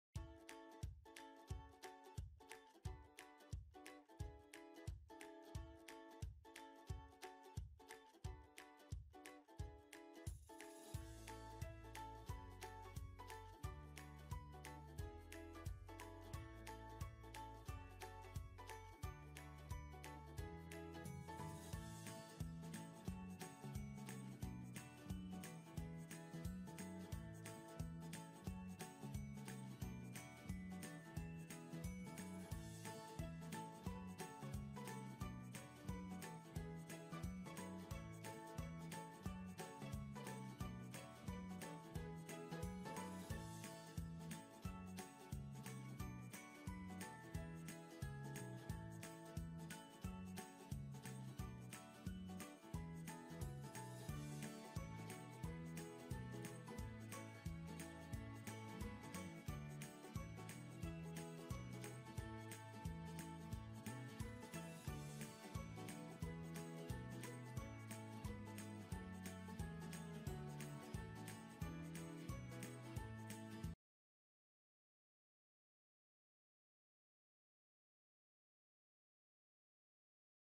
OVS Colle Noire 30 09 2017
Marche dynamique à la colle noire organisée par  Casadelsol